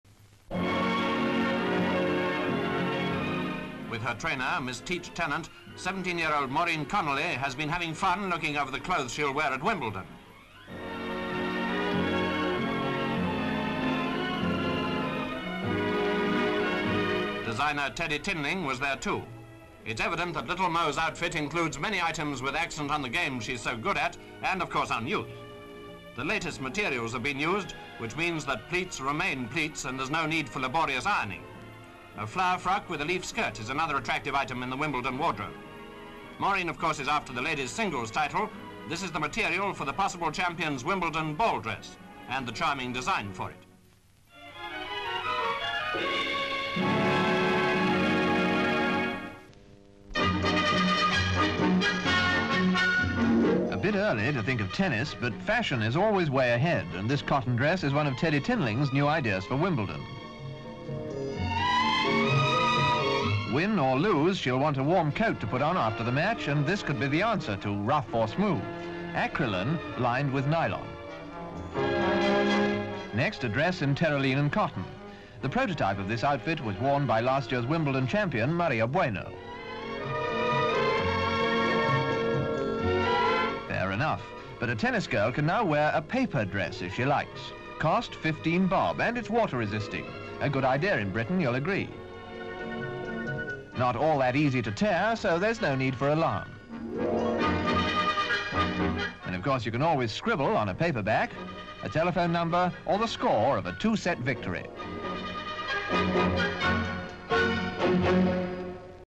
Unusual tennis fashion trends of the past
UK WIMBLEDON FASHION

SOURCE: BRITISH MOVIETONE

RESTRICTION SUMMARY:

LENGTH: 1:48

BRITISH MOVIETONE
Location not supplied - 23 June 1952
++4:3/BLACK AND WHITE/VOICEOVER AND MUSIC FROM SOURCE++
1. Slate reading (English) "Little Mo's Wimbledon Wardrobe"
2. Various of US tennis player Maureen Connolly, aka 'Little Mo' and her trainer Miss "Teach" Tennant looking at clothes Connolly will wear for Wimbledon championships
3. Mid of Connolly with designer Teddy Tinling
4. Various of Connolly wearing outfit with tennis racquet motif
5. Various of Connolly wearing pleated skirt
6. Connolly wearing another skirt
7. Connolly and Tinling looking at material
8. Close of Connolly holding design for ballgown

BRITISH MOVIETONE
Location not supplied - 12 January 1961
9. Slate reading (English) "Tennis Modes"
10. Various of Tinling showing model in one of his tennis dress designs
11. Various of Tinling helping model put on faux fur coat
12. Various of woman modelling dress
13. Three women dresses
14. Close of hand pouring water onto dress
15. Close of hands ripping dress
16. Various of Tinling writing on paper dress

LEADIN:

Tennis fashion is big business, with sports brands vying to sign up the top players to wear their clothes.

But decades ago, another designer had more eccentric ideas of what should be seen on Centre Court.

STORYLINE:

US tennis player Maureen Connolly looks through a whole trunk full of outfits with her coach, Eleanor "Teach" Tennant.

In 1951, she had won the US Open at the tender age of 16, the youngest woman to do so.

A year later, Connolly was gearing up for Wimbledon, one of the most prestigious championships in the tennis calendar.

And designer Teddy Tinling wooed her with his creations.

From racquet motifs, to pleated skirts, the outfits were all in the famous Wimbledon white dress code.

There was even a sketch for a ballgown Connolly could wear to the winners' ball.

The young player did go on win the Wimbledon title that year. Connolly secured nine major singles titles during her career.

In the 1960s, the players had changed, but Tinling was still designing for Wimbledon.

As well and the familiar white dresses, he even suggested a very glamorous faux fur coat for players coming off court.

A more wacky idea was a paper dress.

It was water resistant - perhaps necessary for the English climate.

And it would be written on too.